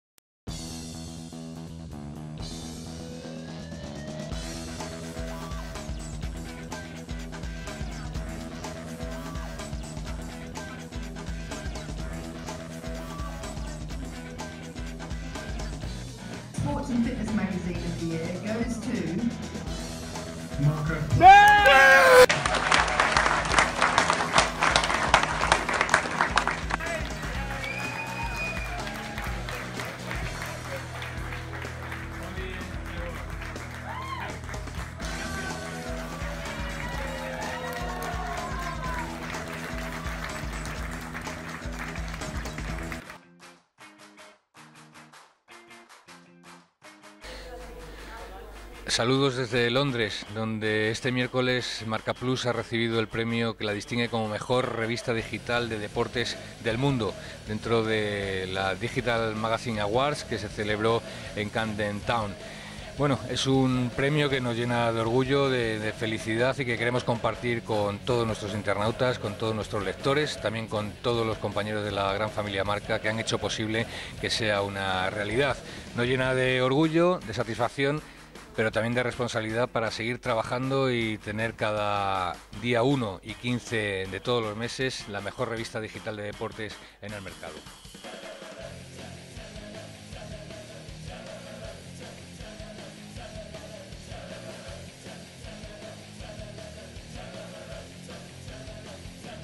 Así fue la gala de premios en Londres
Juan Ignacio Gallardo y Nacho Labarga recogieron el premio de MARCA Plus en la ceremonia celebrada en Inglaterra.